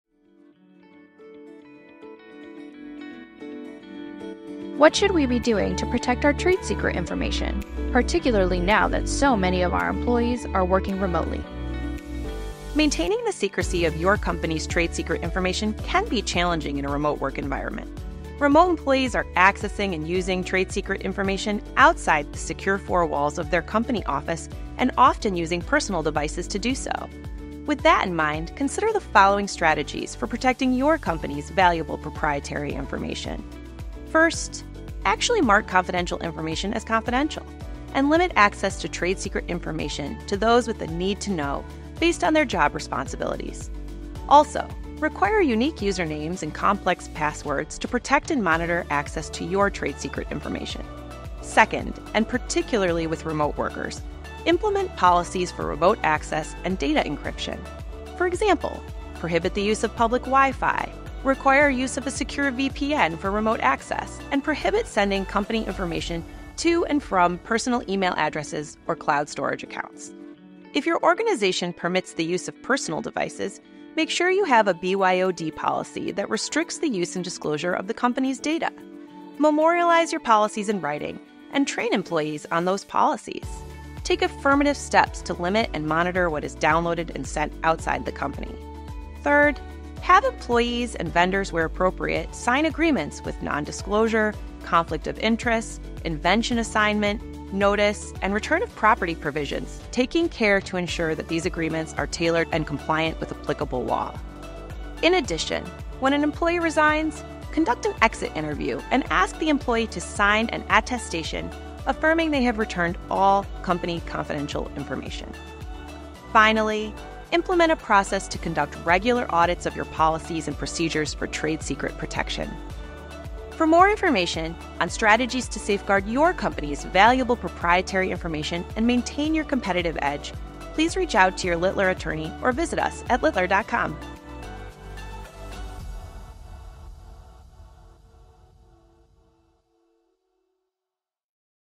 What should we be doing to protect our trade secret information, particularly with remote workers?
In the latest Littler 2 the Point, Jessica Pizzutelli discusses the steps employers can take with their remote employees to ensure valuable proprietary information is protected.